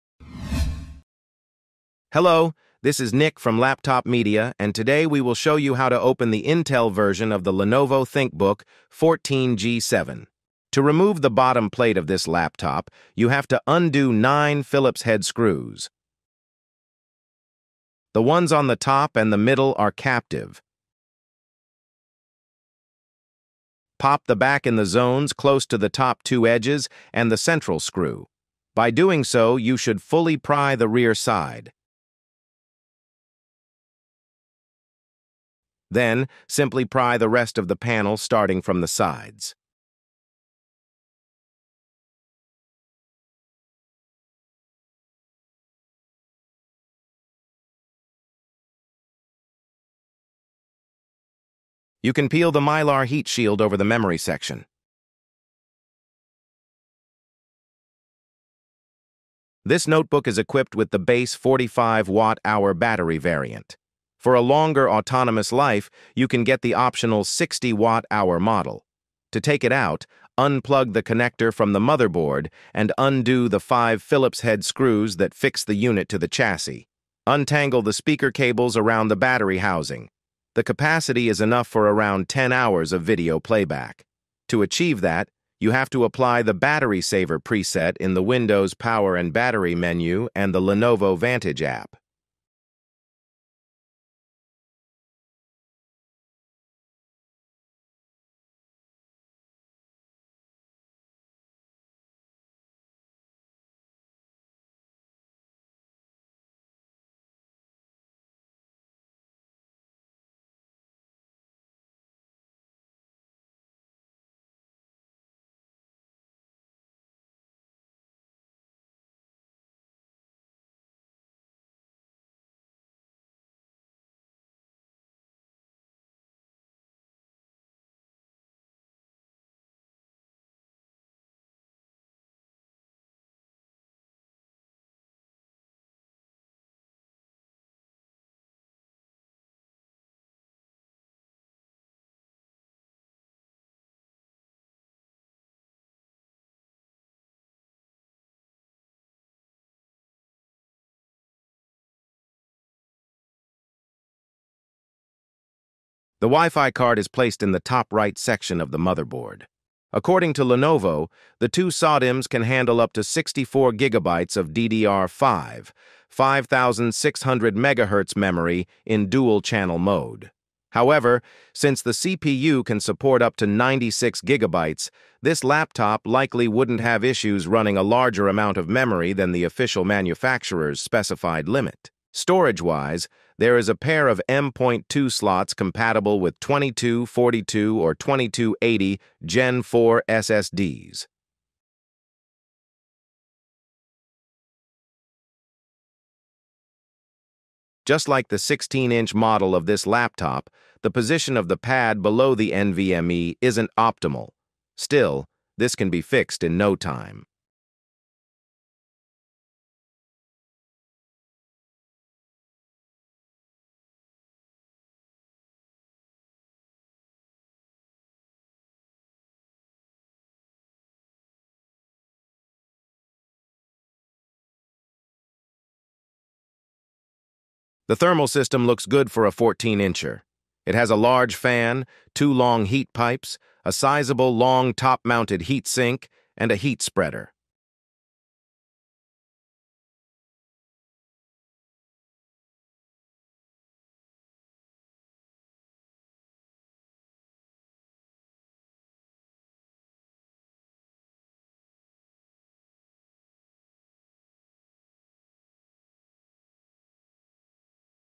🛠️ How to open Lenovo ThinkBook 14 G7 (Intel) - disassembly and upgrade options
✅  LaptopMedia.com